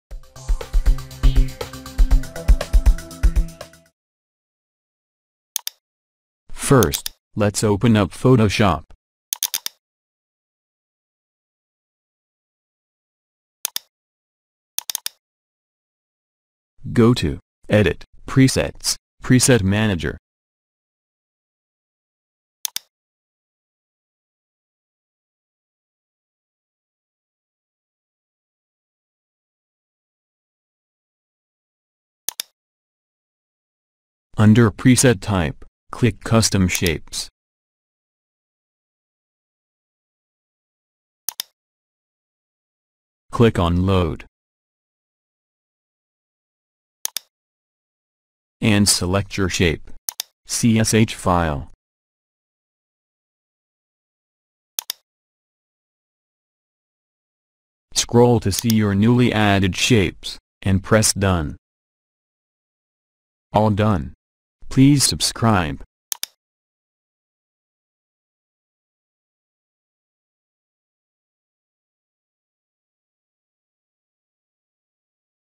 How to install shapes Photoshop CC shapes file (CHS file) - Tutorial
In this tutorial we will show you step by step how to easily install Photoshop custom shapes (.chs files) into Photoshop CC, this tutorial was made in Adobe Photoshop CC 2018.

What is Photoshop CSH file format?

Photoshop Custom Shapes are handy-dandy little things to have in your design arsenal. A Custom Shape, or ".csh" file is a collection of graphics that you can use to draw shapes with in Photoshop.